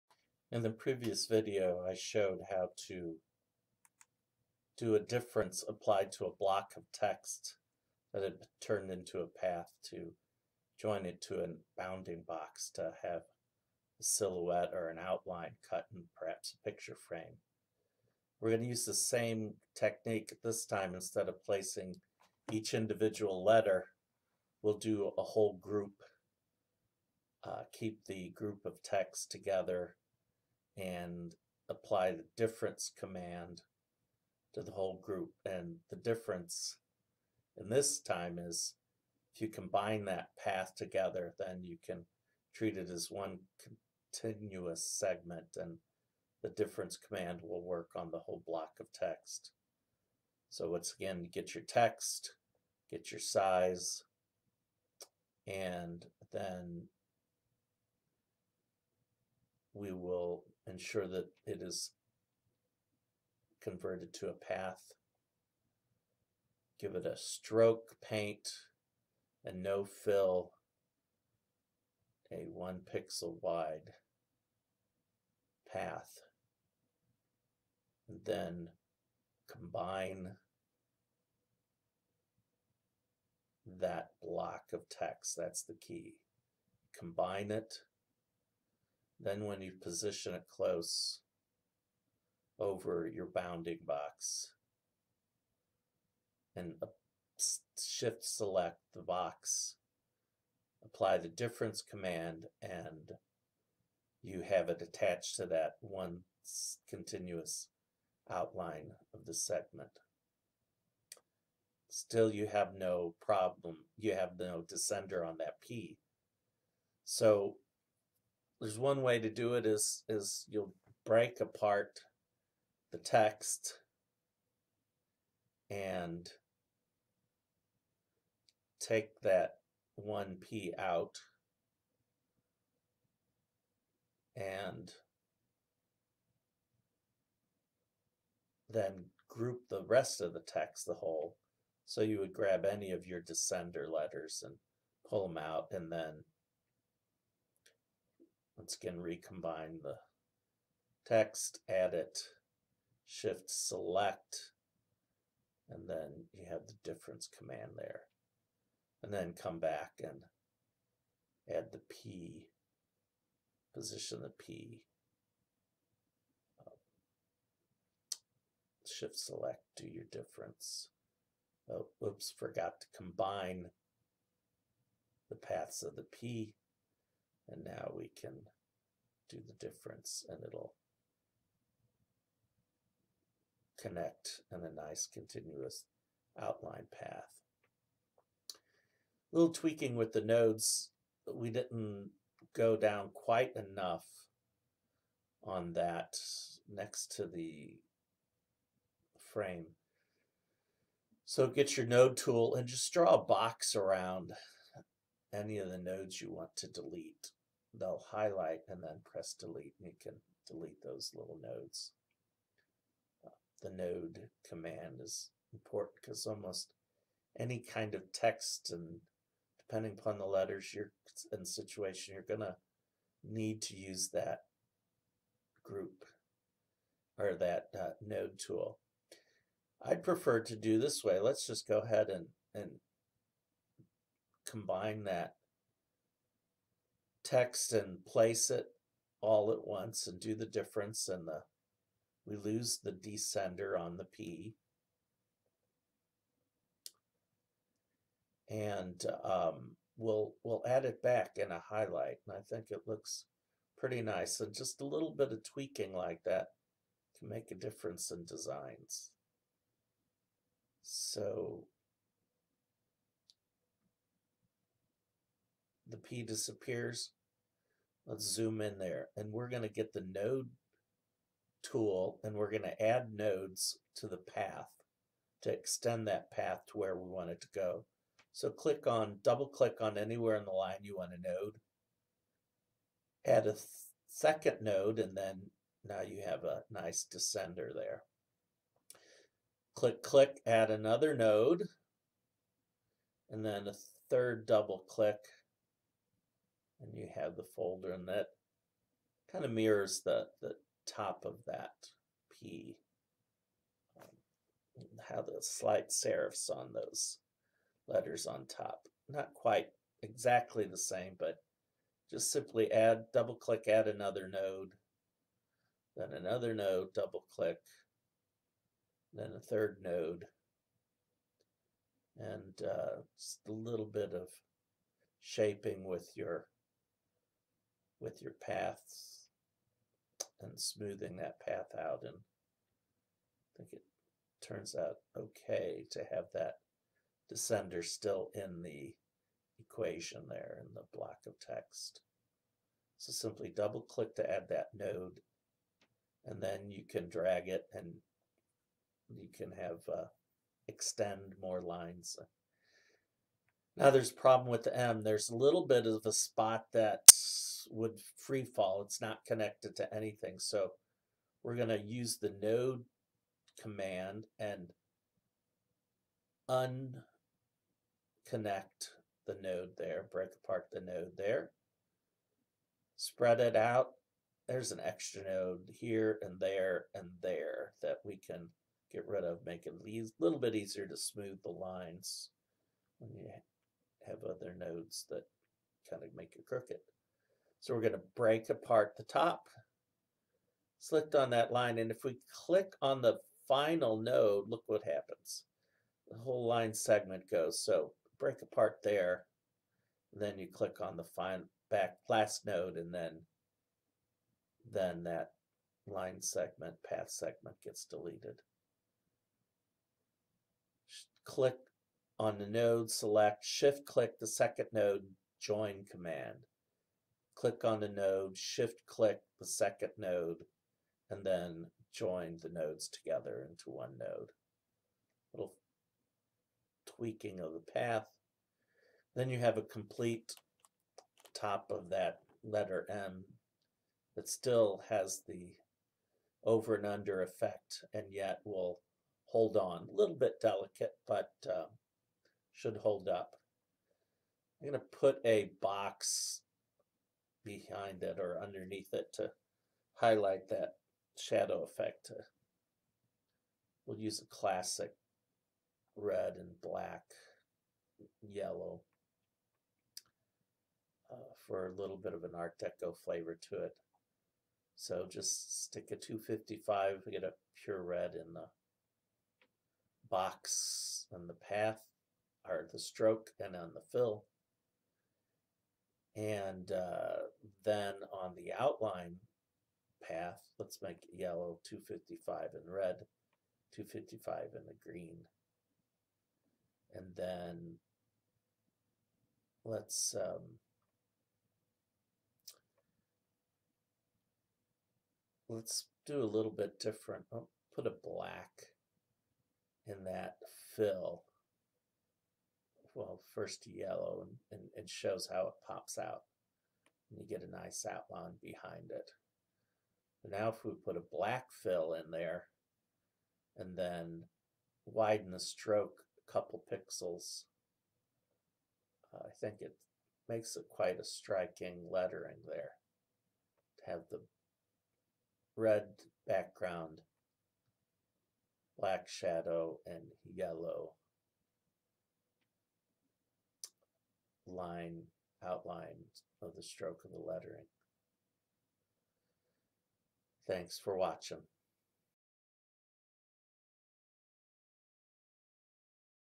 Inkscape Tutorial: using the difference command to join paths
When multiple paths are combined, you can use the difference command to make one continuous outline path. Demo with a font.